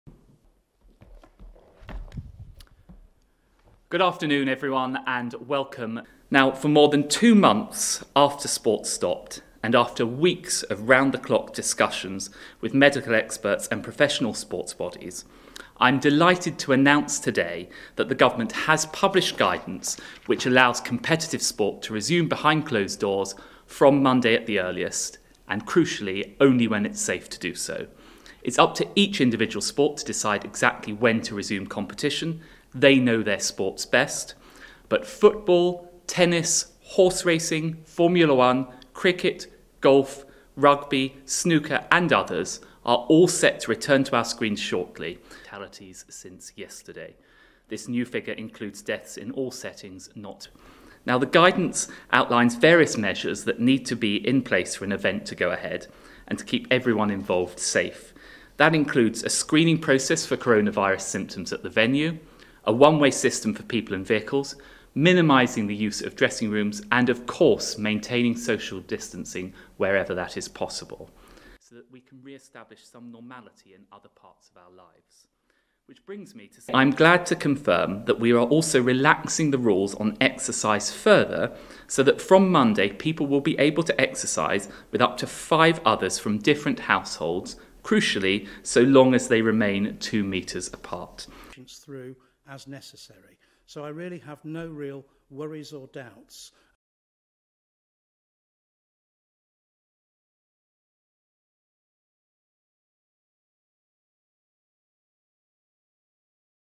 Coronavirus: British competitive sports given green light for June 1 restart
After a near three-month shutdown, all competitive sport in Britain can resume from June 1 behind closed doors provided strict conditions are met, the government said on Saturday.

No competitive top-level sport has taken place in Britain since March as the country deals with the coronavirus pandemic that has killed more than 38,000 people in the UK.

In a document published on Saturday by the Department for Digital, Culture, Media and Sport (DCMS), Culture Secretary Oliver Dowden outlined the strict criteria that must be met to allow elite athletes and professional sportsmen to resume.

Competitive sport would be able to resume behind closed doors and only when it was safe to do so, he said during a daily Downing Street briefing, adding that it was up to each individual sport to decide exactly when to restart.

The government's stage three guidance for staging competitive sporting events includes conditions such as players travelling alone to venues, being screened for coronavirus symptoms and maintaining social distancing where possible.

Dowden also confirmed that the government was relaxing rules on all forms of exercise by allowing up to five people from different households to train together from June 1.